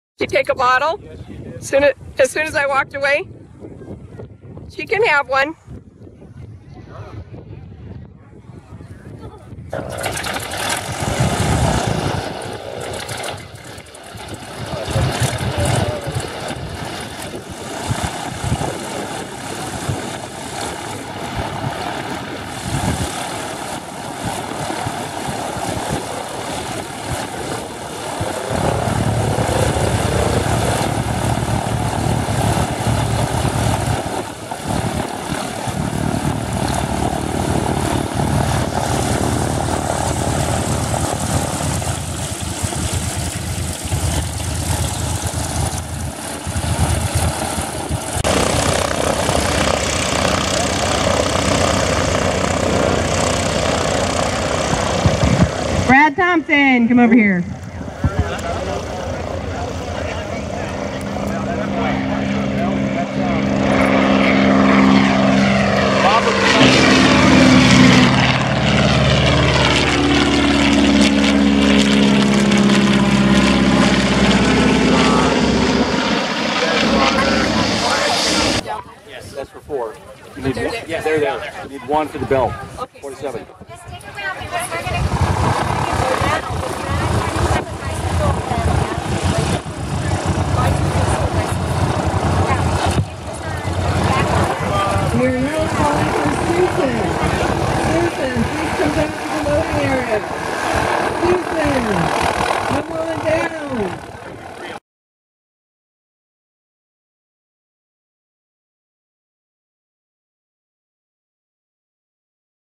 Flight in WACO Biplane at Agua Dulce Airport N369BP
Recorded on June 28, 2014. My wife Gail Whisenant shot video of our friend John Pedersen taking a flight in this 1987 Classic Aircraft Corp WACO YMF-5 "Spirit of Agua Dulce" (Based on a 1934 WACO) at the Agua Dulce Airport Community Fundraiser Presented by: Agua Dulce Airport Association